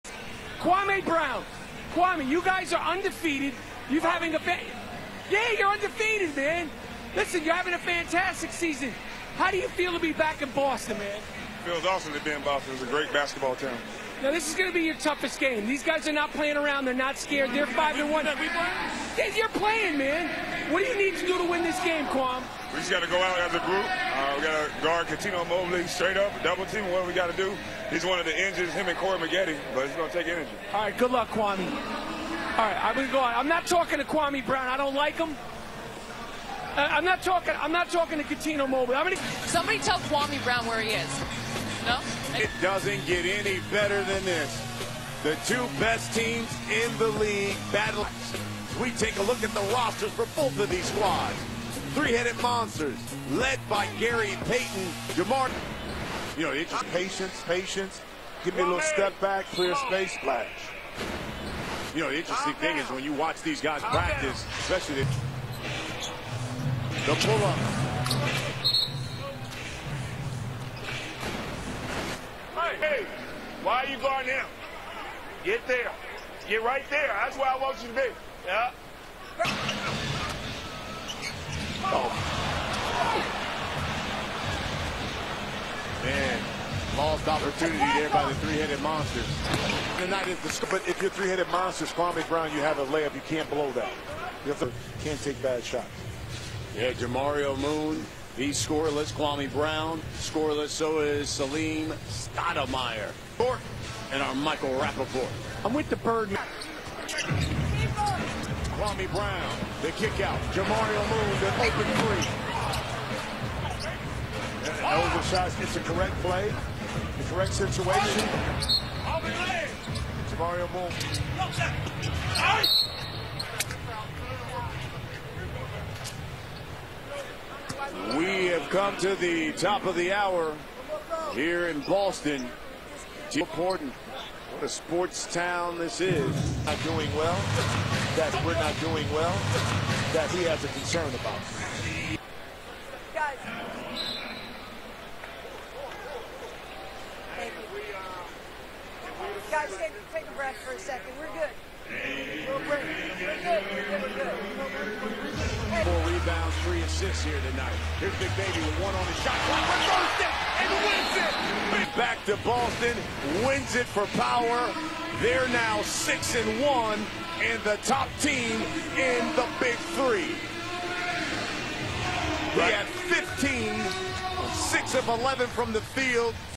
Kwame Brown Worst Game. He Didn't Even Know If He Was Playing... BIG3: S2G7
Obviously second sentence from the interview and it is a joke but it sounds real to me right know. Terrible game from Kwame and Monsters. Kwame didn't do what he always do and never even looked at a rim. He played solid period of time and only managed to grap couple boards and assists. Missed a wide open putback.

Monsters lost to Power 51-40 so they will battle for the 1st seed. Both teams have 6-1 record.

Next week they're playing in Kwame's Motherland - Atlanta. Let's see what will happen'...